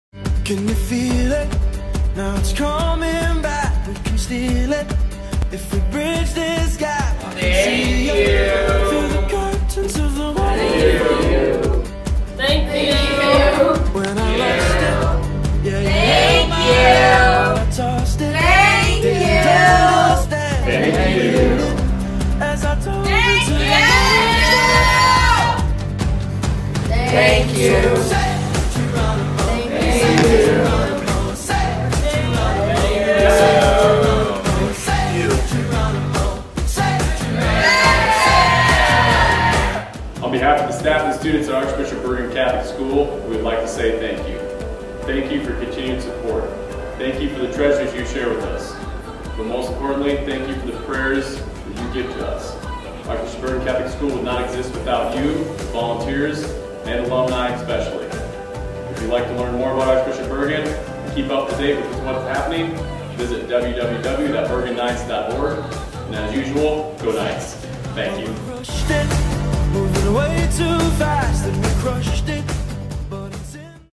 Thank You for Your Contribution to the Bergan Fund
Archbishop Bergan Catholic School wants to thank donors who participated in the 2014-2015 Bergan Fund, especially those who helped us raise our last $15,000 in 24 hours with our Day of Giving Online.